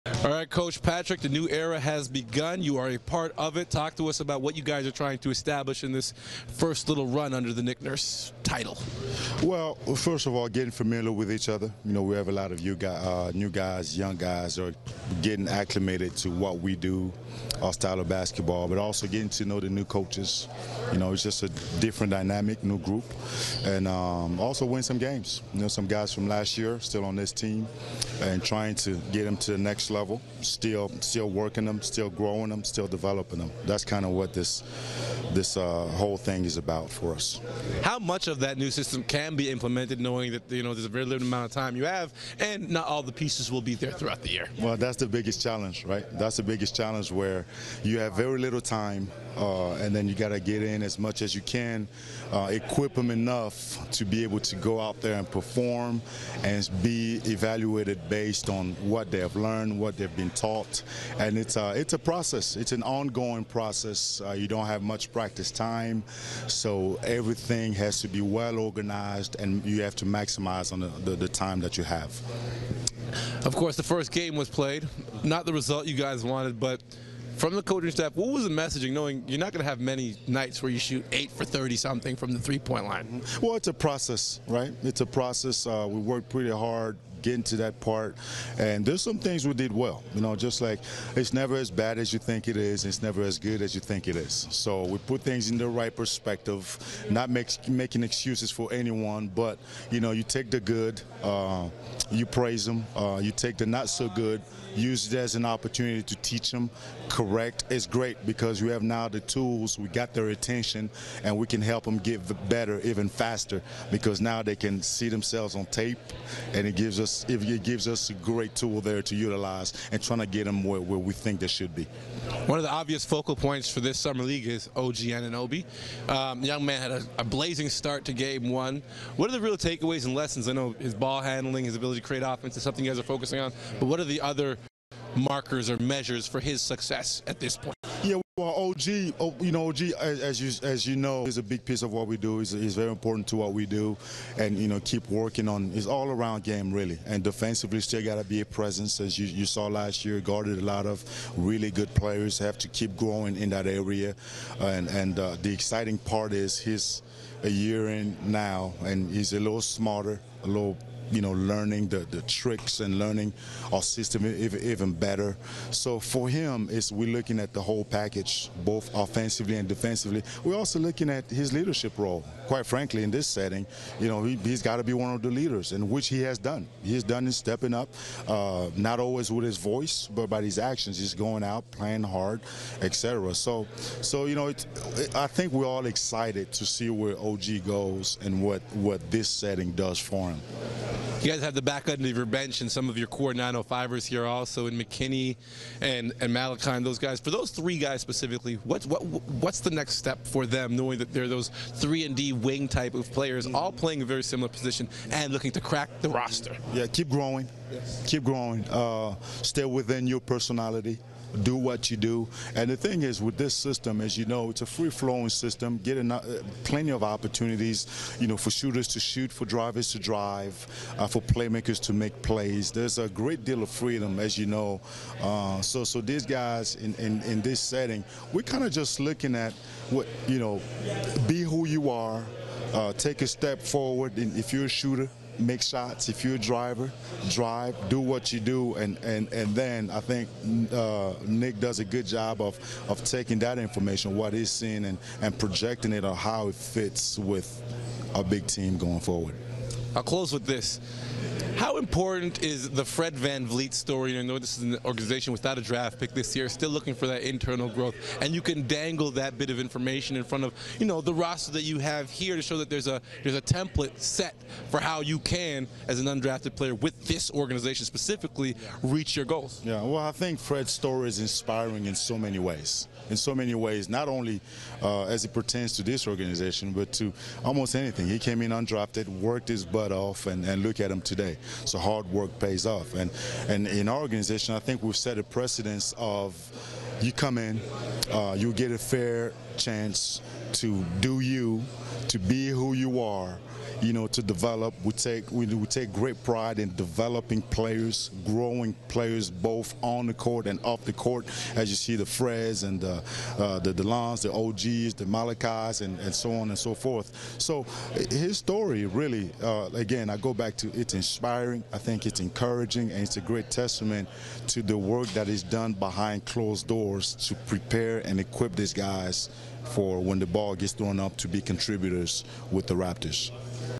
Raptors Summer League: Patrick Mutombo - July 7, 2018
Patrick Mutombo speaks with NBA TV Canada following practice on Saturday.